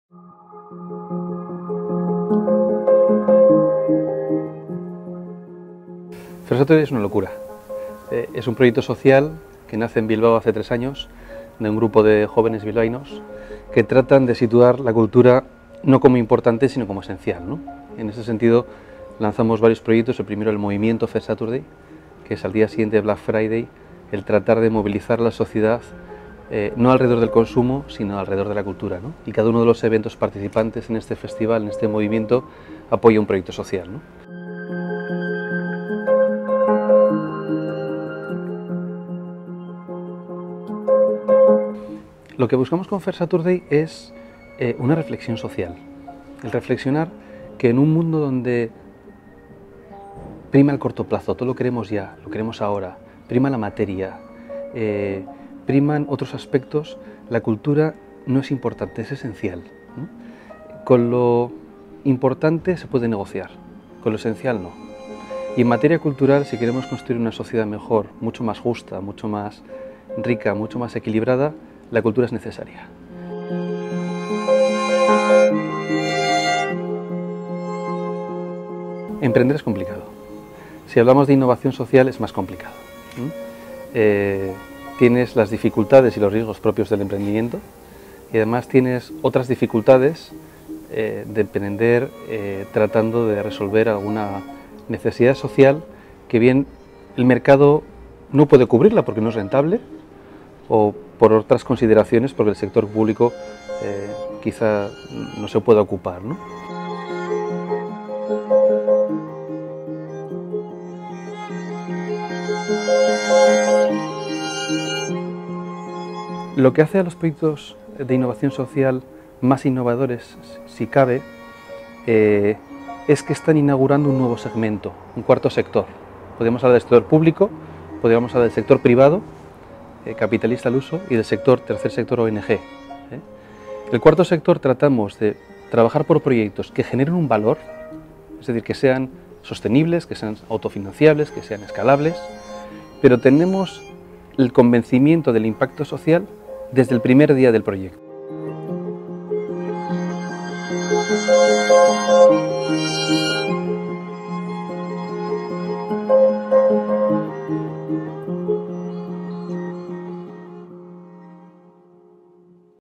Fair Saturday
Jordi Albareda nos comenta el proyecto de Fair Saturday desde el punto de vista de la innovación social